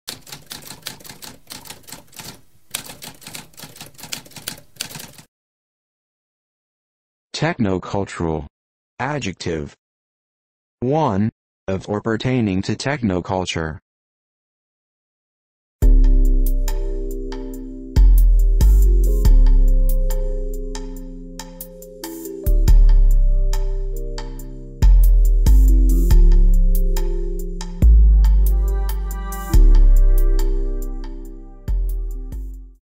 What does technocultural mean?
A spoken definition of technocultural.

Intro Sound:
Typewriter - Tamskp